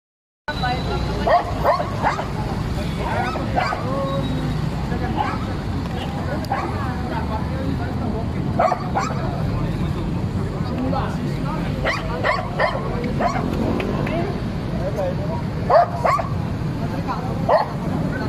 CUTE ALASKAN MALAMUTE Spotted
This is Phoenix, a one year and 5 months old Alaskan Malamute that I met in Esplanade 4 here in Iloilo City.